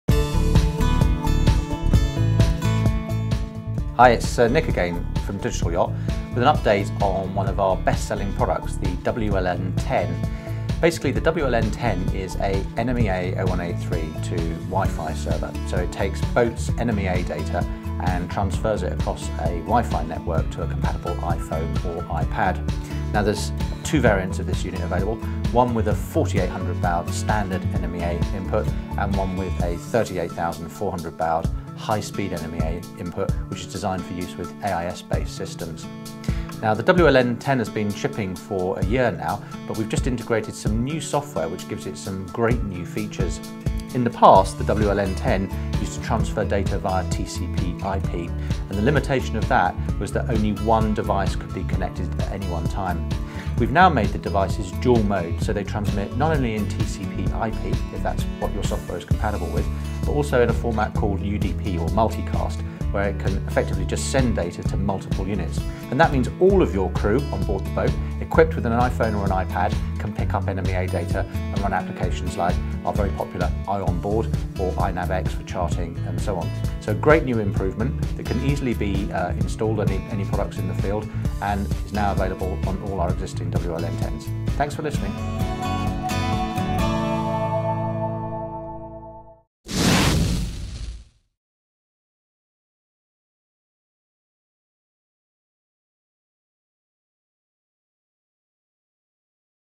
WLN10 - Get NMEA data in your iPhone or iPad
In this short video, CEO, Nick Heyes, explains about a recent software update which means your boats NMEA data can now be sent to multiple iPhone and IPad units.